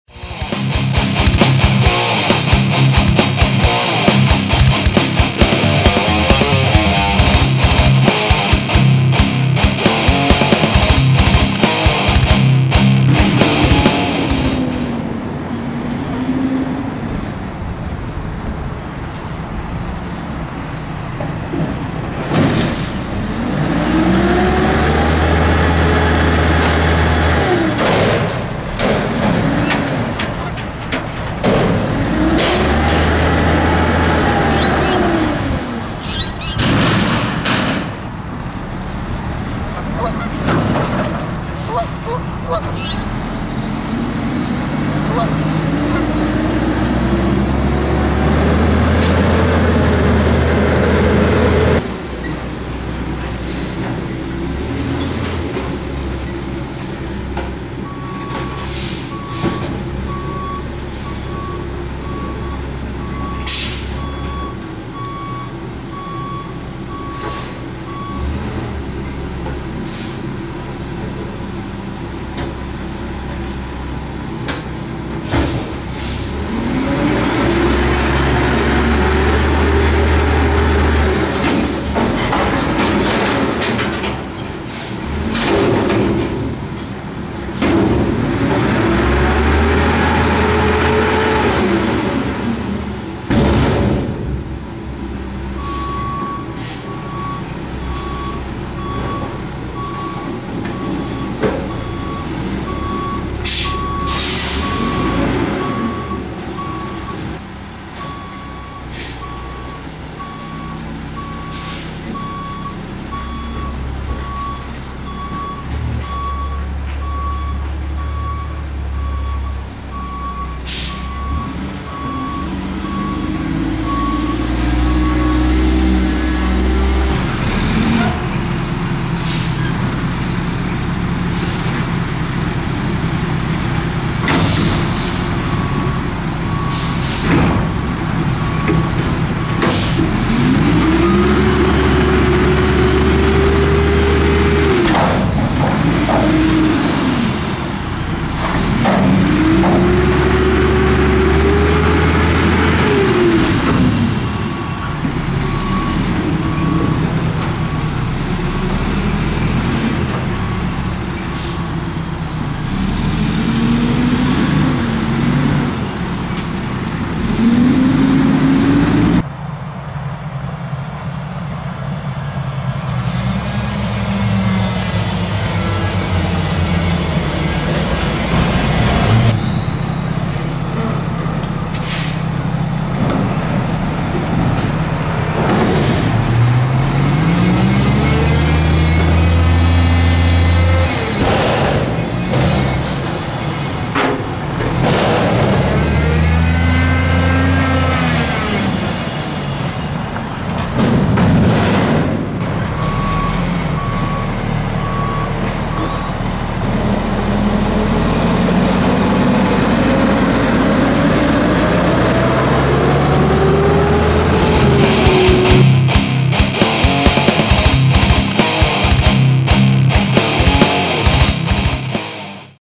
Mack MR - McNeilus Pacific Series Front Load Garbage Truck
I know it's a short video, but this truck was rather elusive! Here we have a McNeilus Pacific Series front load body on a Mack MR cab and chassis, slamming back some dumpsters around town! I rarely get the opportunity to film commercial front loaders, as the routes tend to be a lot harder to track down than residential routes, so I was very excited to be able to catch this truck in action! I hope you guys enjoy this brief footage, too!

I'd like to extend a big thanks to the driver of this truck for providing our community with excellent service!

Theme Music Written, Composed, and Recorded by Thrash 'N' Trash Productions